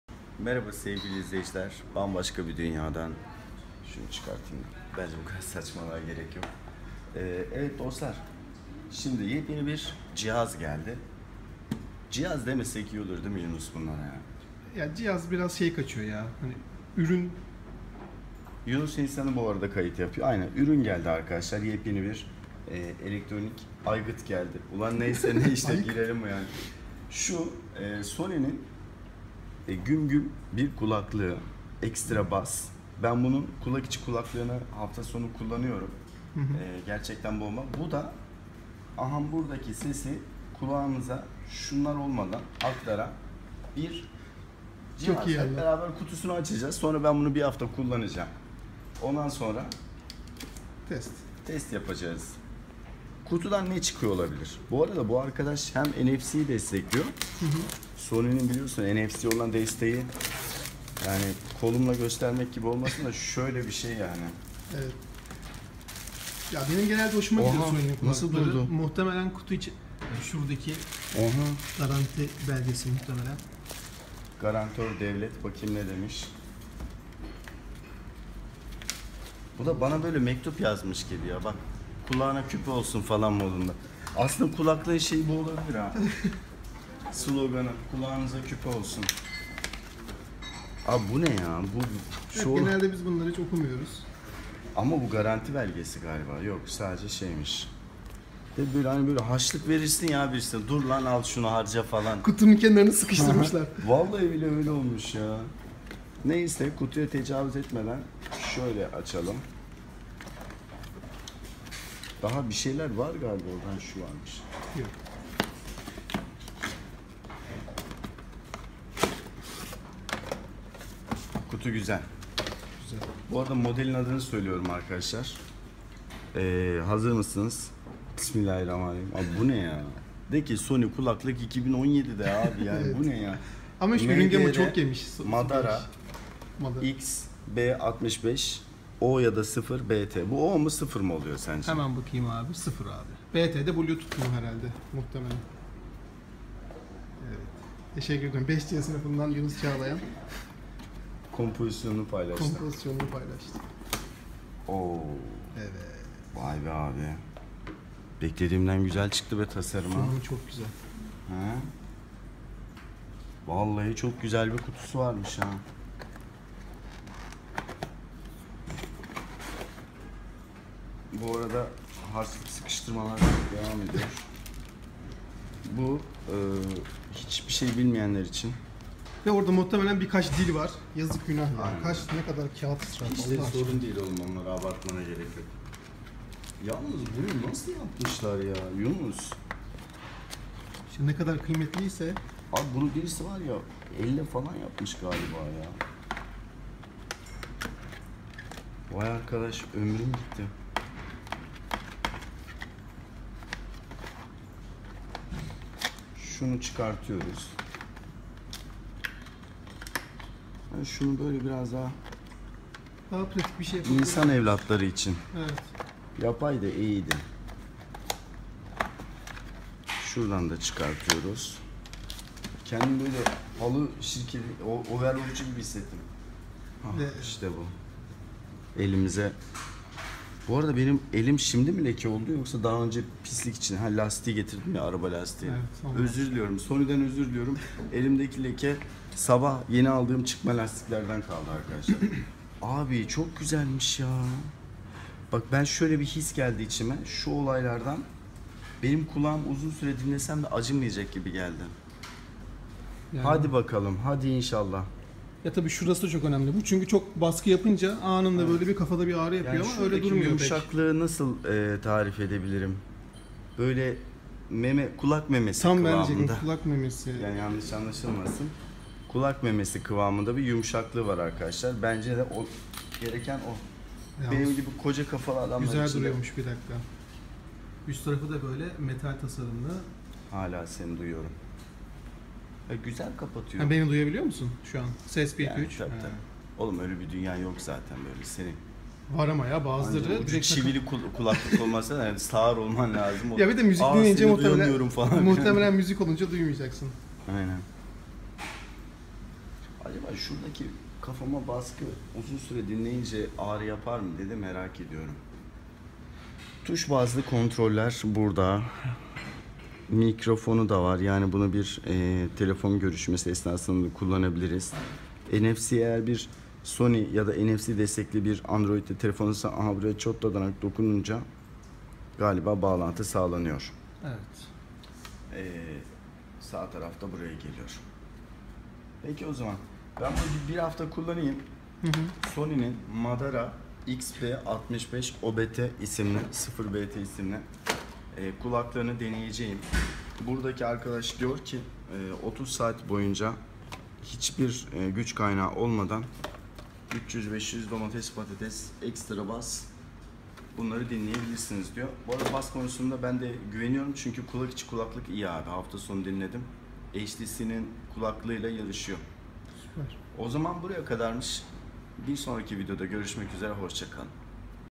Bass'larıyla İnleten Kablosuz Kulaklık Kutusundan Çıkıyor! (Sony MDR-XB650BT)
Kulaklık pazarının en önemli oyunlarından biri olan Sony, her geçen gün yeni modellerle tüketicilerin karşısına çıkıyor. Bu videomuzda Extra Bass etiketine sahip Sony MDR-XB650BT kablosuz kulaklığın kutu içeriğine bakıyoruz.

Sony MDR-XB650BT Özellikleri

Kırmızı, mavi ve siyah olmak üzere üç farklı renk seçeneği ile gelen Sony MDR-XB650BT, hem NFC hem de Bluetooth destekliyor. Bu sayede ürünü mobil cihazlarla veya farklı ürünlerle kolayca eşleştirebiliyoruz.

11.82 in neodymium sürücülere sahip Sony MDR-XB650BT, Bass Booster teknolojisi sayesinde bass beklentisi olan kullanıcılar için ideal bir seçenek. Ayrıca kulaklıkta yer alan dahili mikrofon ile telefon görüşmesi yapılabildiğini de belirtelim.

Son olarak ürünün 30 saatlik bir pil ömrü sunduğunu da belirtelim.